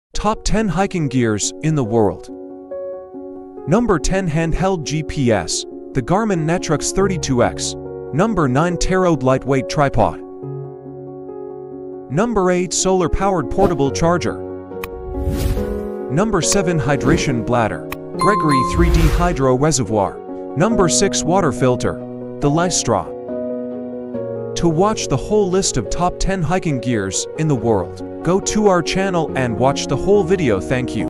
Top 10 Hiking Gears And Gadgets.
Gear up for epic outdoor adventures with our thrilling lineup of the top ten hiking gear essentials! From the Garmin eTrex 32x for navigation to the versatile GoPro 11 for capturing jaw-dropping footage, we've handpicked the gear that will ignite your wanderlust and elevate your hiking experience. Join us as we dive into the world of unbeatable hiking gear that will make you the true champion of the trails. Pack your backpack, embrace the call of the mountains, and get ready to create unforgettable memories in the great outdoors. Stay wild, stay adventurous, and let's embark on an incredible hiking journey together! Happy trails!